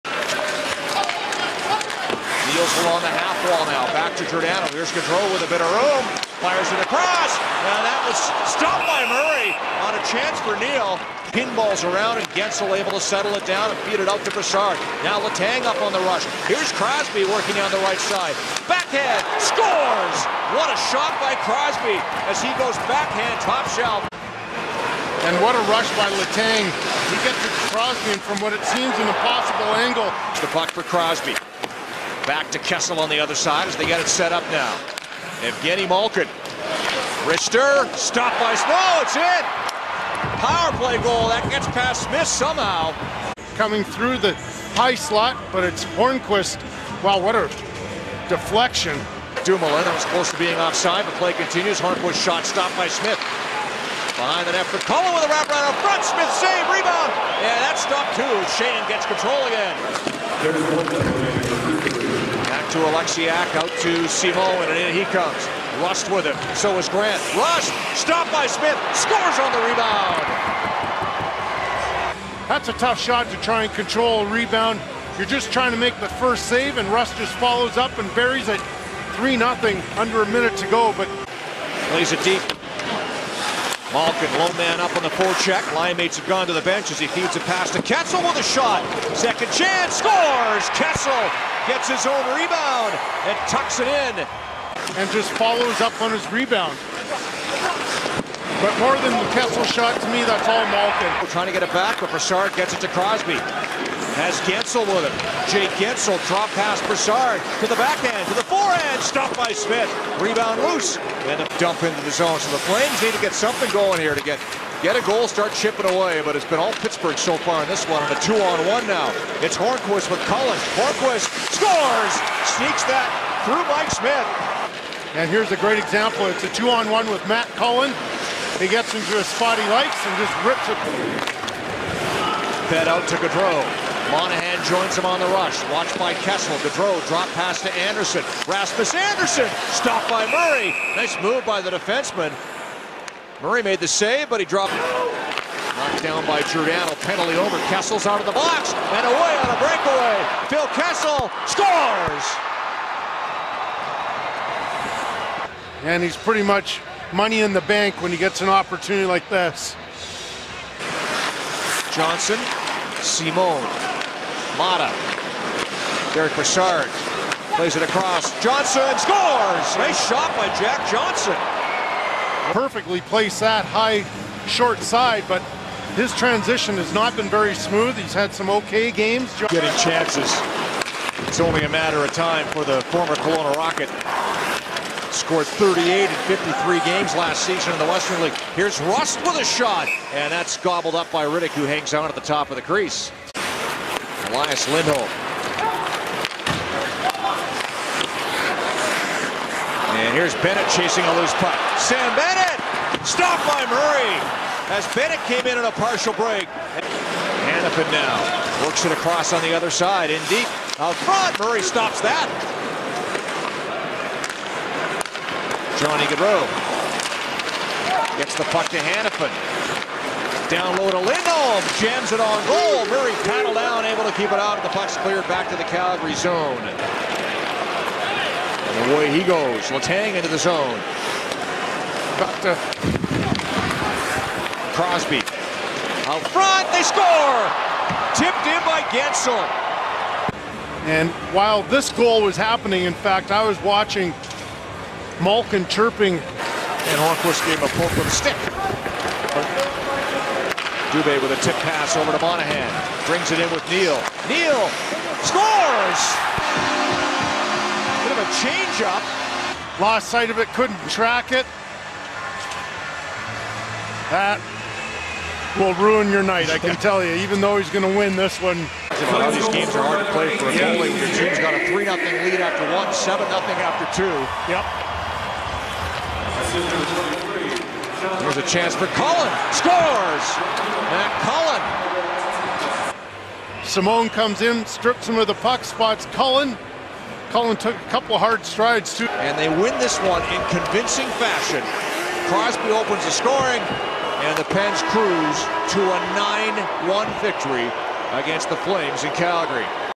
NHL Highlights | Penguins vs. Flames - Oct. 25, 2018
The Pittsburgh Penguins destroyed the Calgary Flames 9-1 on Thursday night at Scotiabank Saddledome.